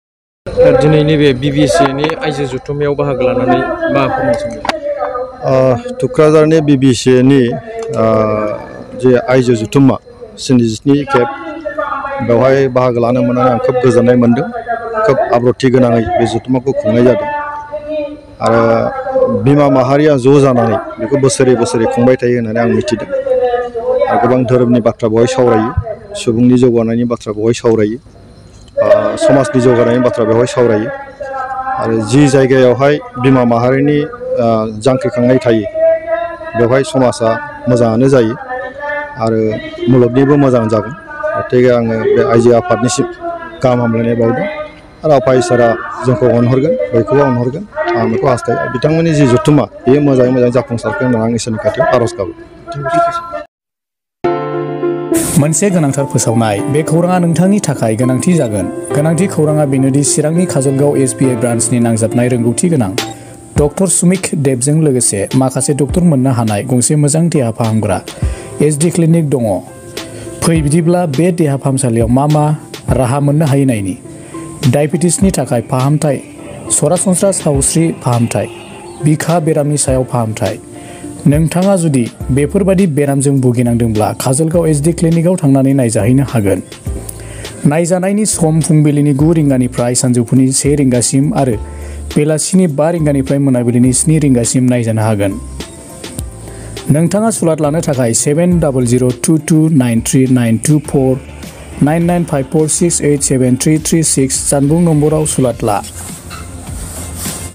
BBCAनि 77थि आयजो आफादनि जुथुमायाव MP रोनगोरा नार्जारीया बाहागो लाना खौरांगिरिनि सिगांआव बिबुंथि
सिरां जिल्ला थुख्राझार BBCA आयजो आफादनि 77 थि बोसोरारि जथुम्मायाव बाहागो लादों MP जयन्त बसुमतारी MP रोनगोरा नार्जारी आरो EM रनजित बसुमतारी मोनहा।